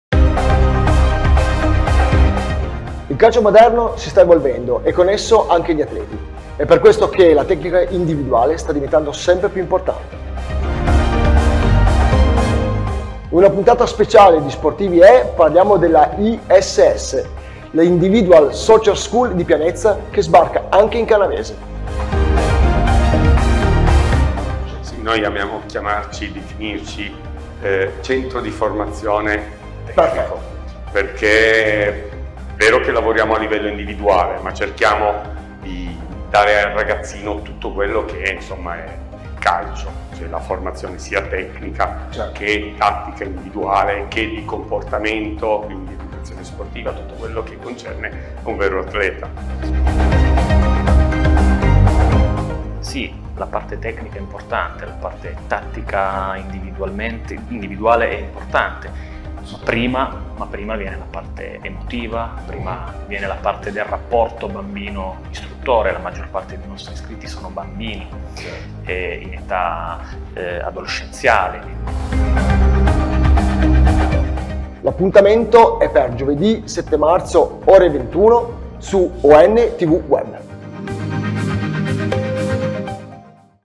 Giovedì 7 marzo - ore 21.00 - SPORTIVI E... - Con la Iss si migliora la tecnica calcistica
Anche in Canavese, grazie alla collaborazione con l'Ardor San Francesco, sbarca l'Individual Soccer School. Il centro di formazione che ha la sua sede centrale a Pianezza ha deciso di portare nella nostra zona il proprio progetto. Per saperne di più seguite l'intervista in onda giovedì 7 marzo 2024 alle ore 21 su